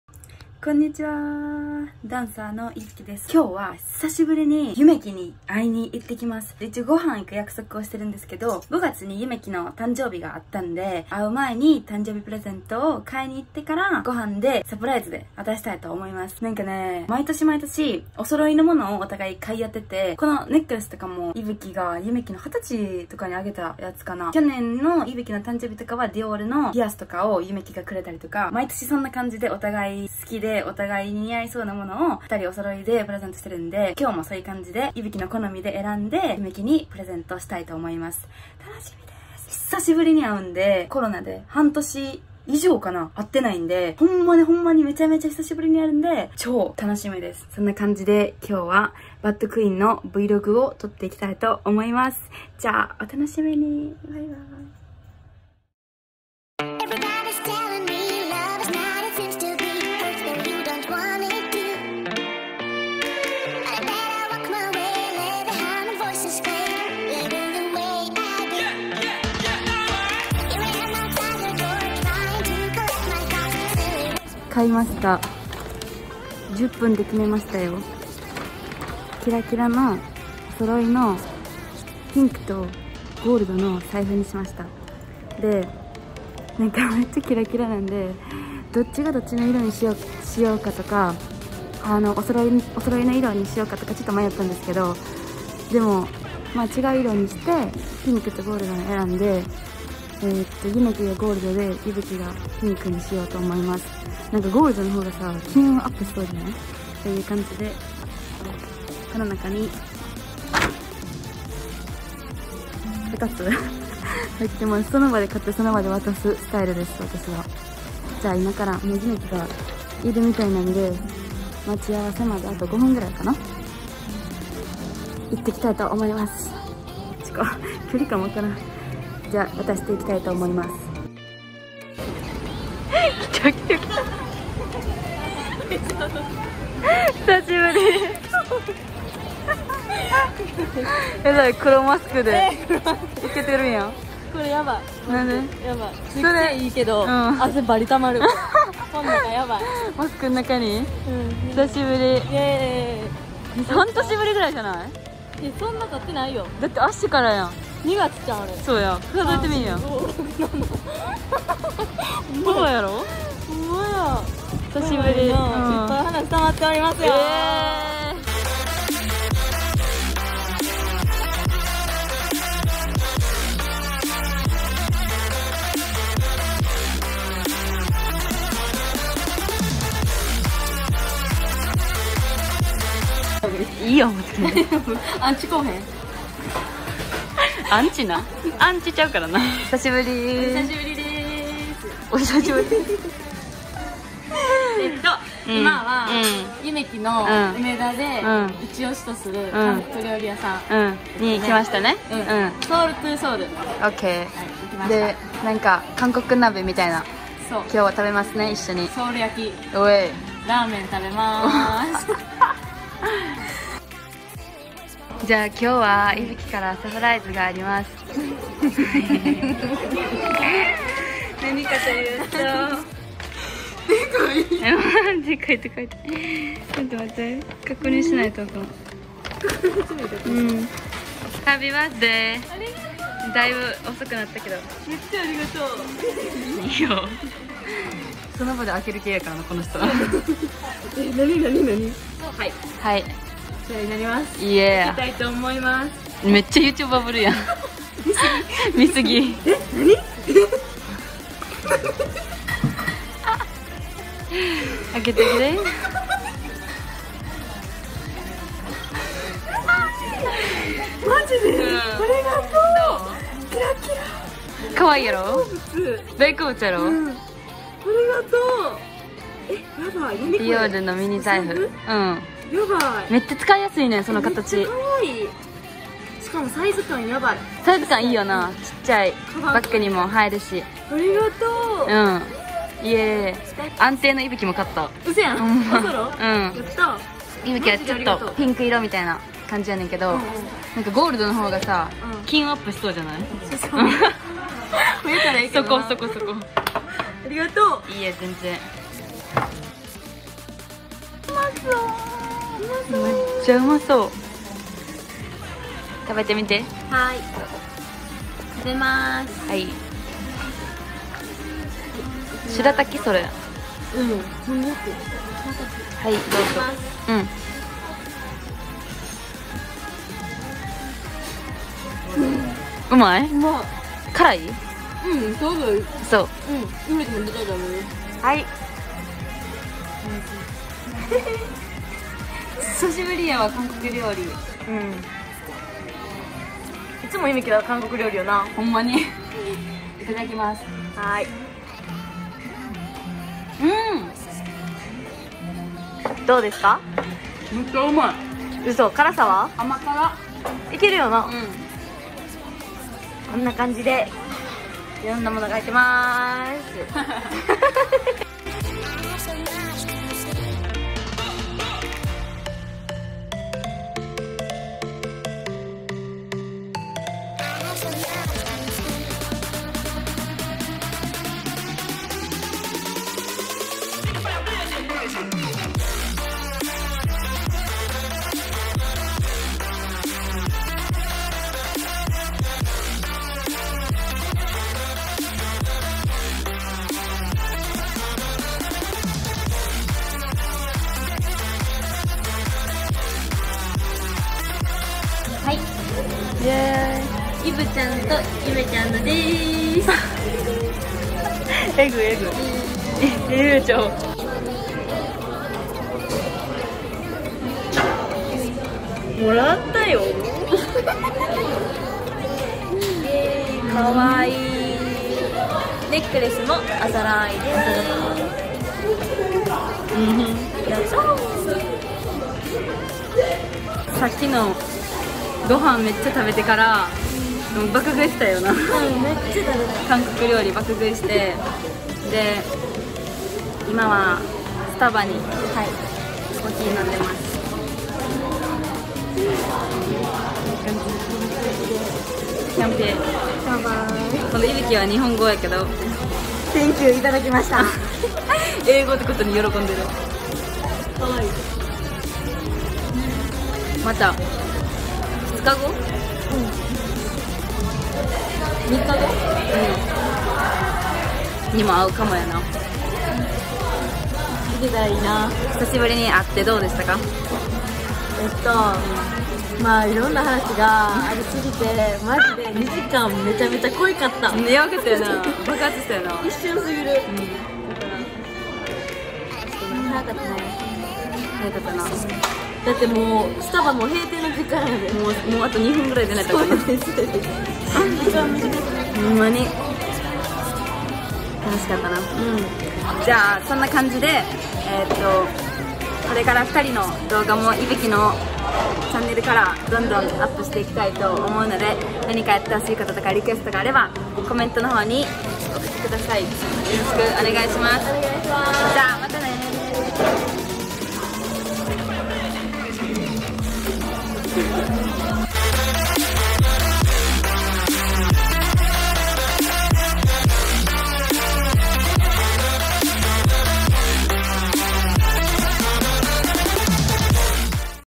【BAD QUEEN】Vlog 久しぶりにYumekiに会いに... 💜
BAD QUEEN 相方Yumekiとの久しぶりの再会✨
Yumekiがお誕生日だったのでBirthday サプライズしてみました✨